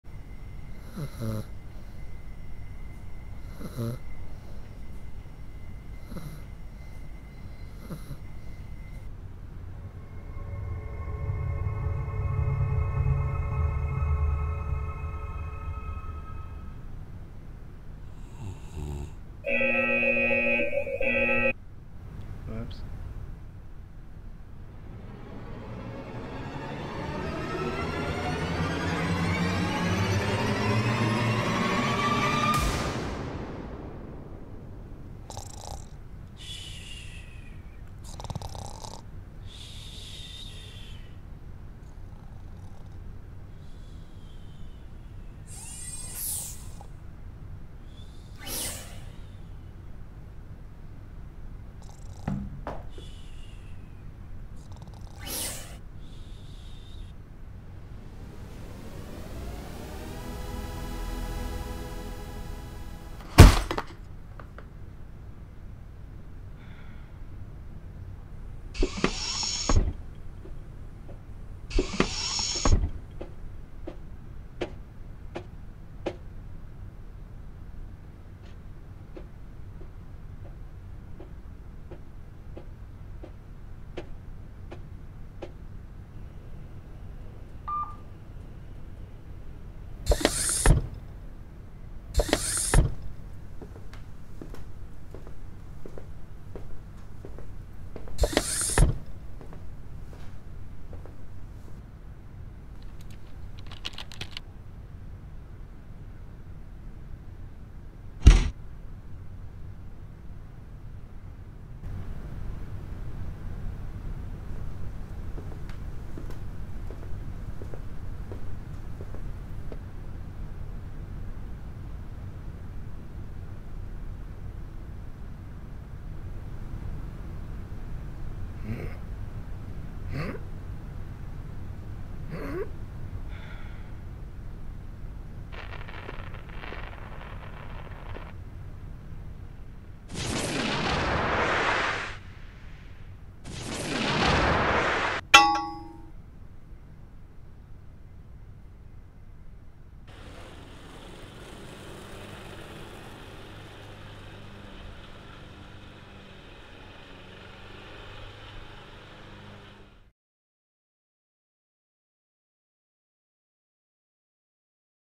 Spaced
3d animatie eindwerk Bart Hansoul voor CVO Leuven